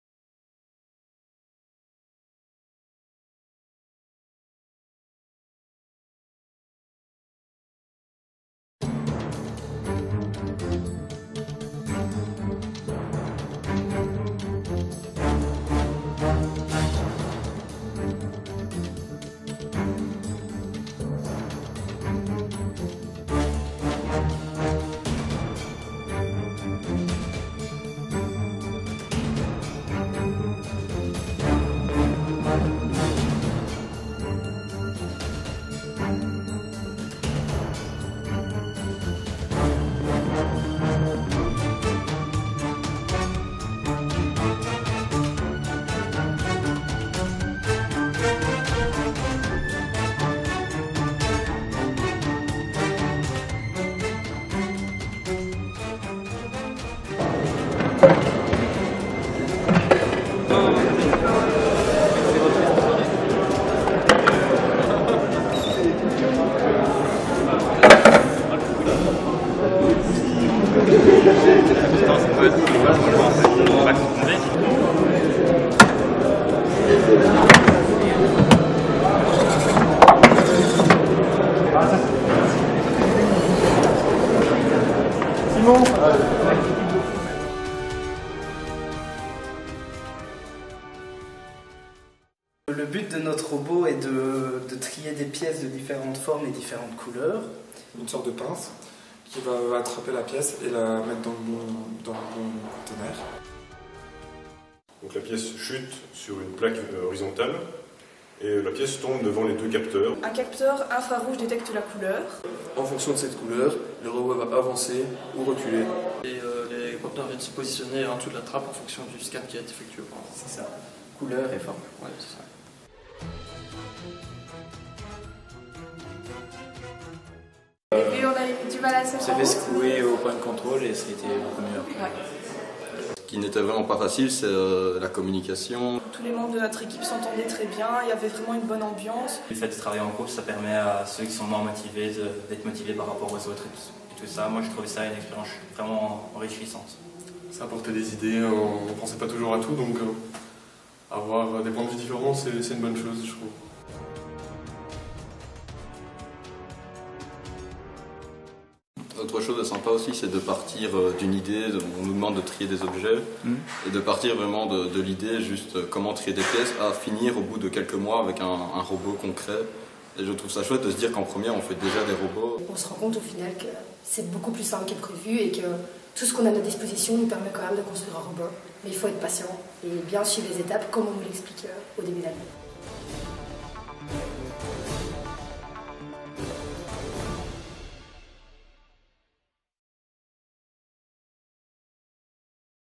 Projet de création d'un dispositif automatisé de tri de pièces radioactives
La Faculté Polytechnique a proposé à ses étudiants de Bac 1 de de travailler sur 1 projet concret : réaliser un dispositif automatisé de tri de pièces radioactives.

Philippe FORTEMPS
Université de Mons
Faculté polytechnique
Rue de Houdain, 9
B-7000 Mons
Belgique